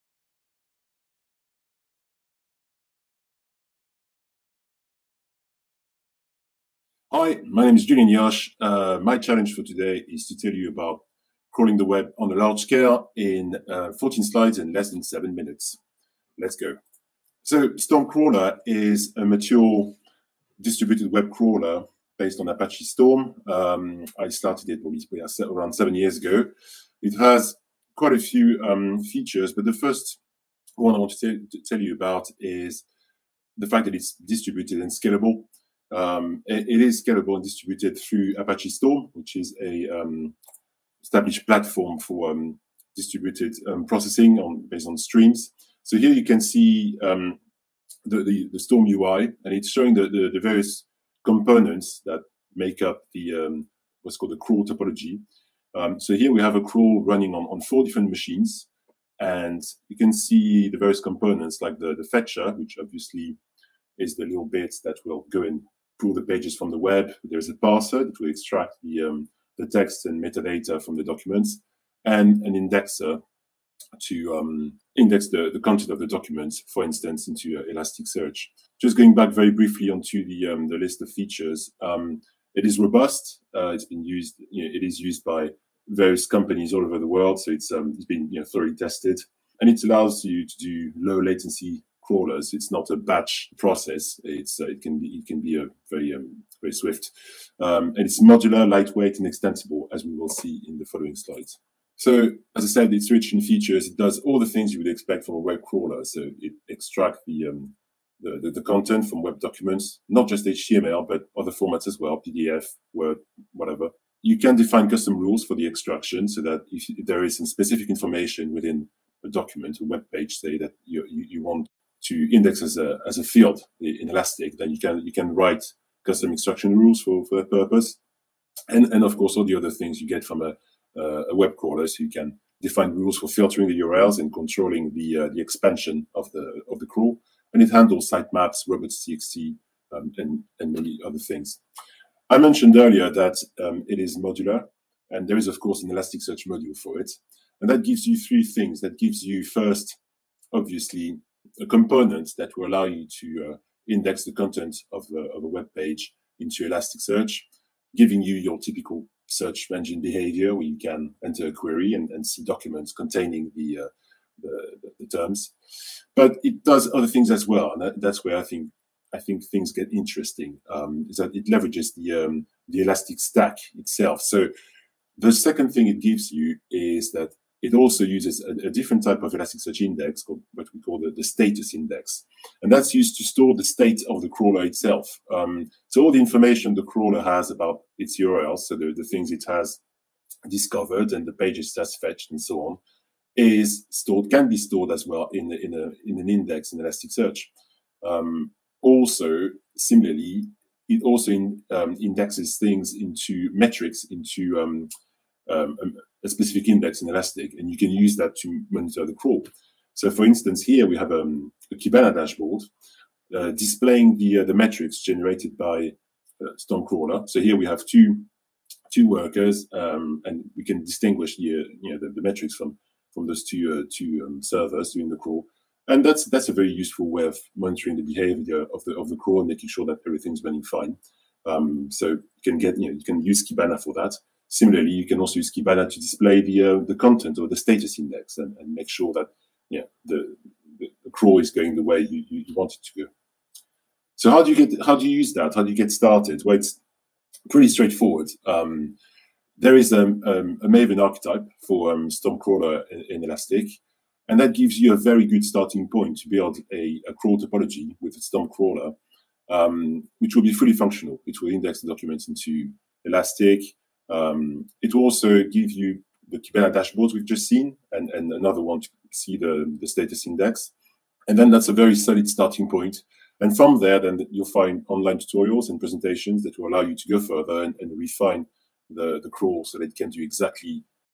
ElasticCC: Crawl the Web on a Large Scale with Stormcrawler and Elasticsearch
By Julien Nioche

StormCrawler is a popular and mature open-source web crawler. It is written in Java and is both lightweight and scalable, thanks to the distribution layer based on Apache Storm. One of the attractions of the crawler is that it is extensible and modular, as well as versatile. In this presentation, we will have a closer look at the Elasticsearch module of StormCrawler and see how it is being used in production by various organizations, sometimes on a very large scale.